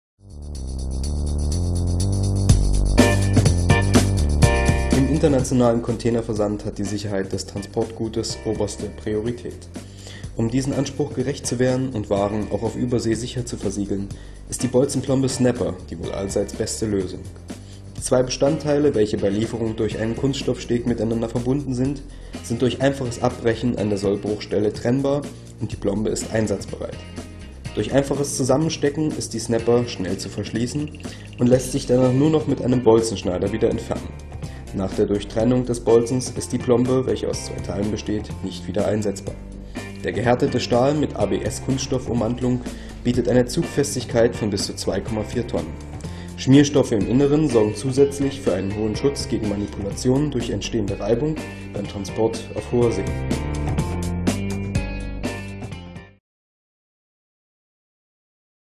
Containerplombe Snapper
Die Containerplombe Snapper ist optimal für den Export, da sie den höchsten Sicherheitsrichtlinien entspricht.  Zollzulassung C-TPAT und ist ISO/PAS 17712:2013 zertifiziert. Zum Produkt

Sicherheit und zuverlässige Warenlogistik ist oberstes Gebot. PLOMBEN-SHOP.de bietet Containerplomben und Containerschlösser für Überseecontainer & Export: